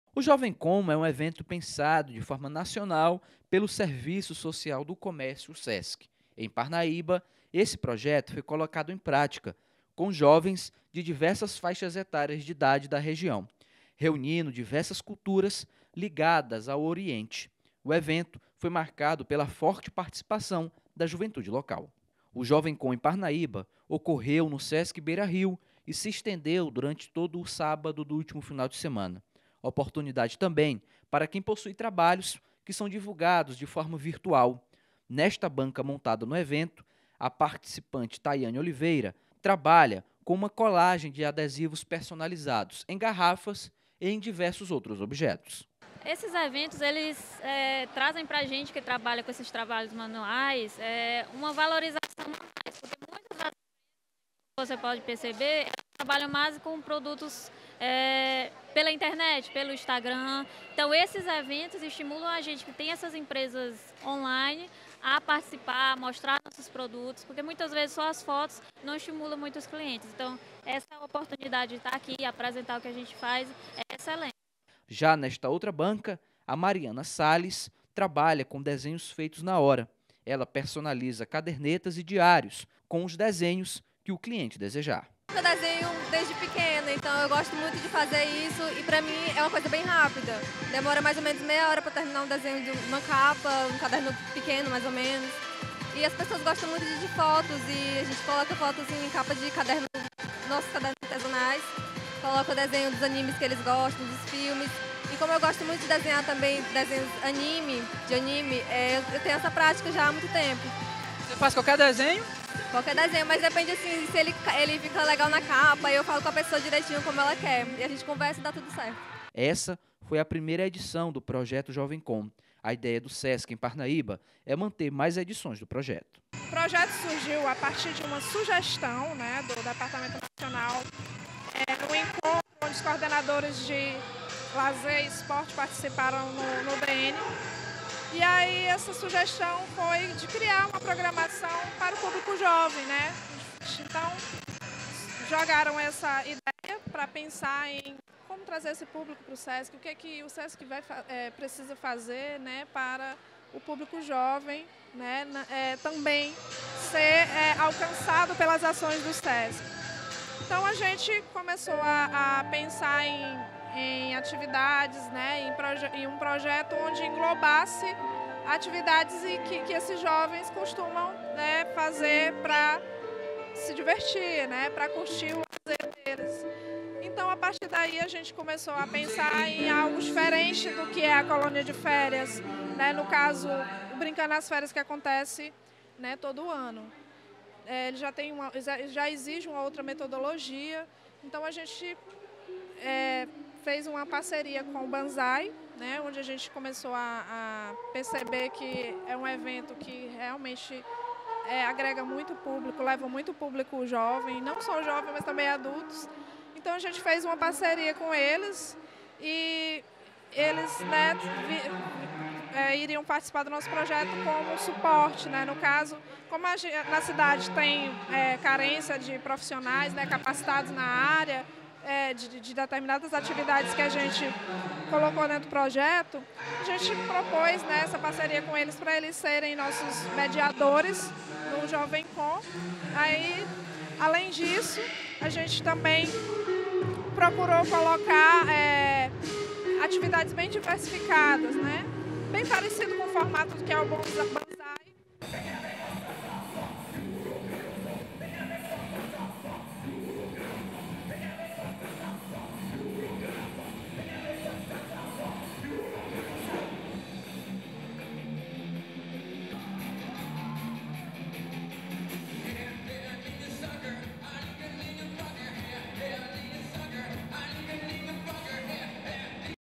TV Delta: Parnaibanos prestigiam o evento JovemCon
Com o apoio do Instituto Federal do Piauí e Banzai, o Sesc realizou em Parnaíba, o evento JovemCon, que desenvolveu diversas atividades voltadas para a cultura jovem. Confira mais detalhes na reportagem!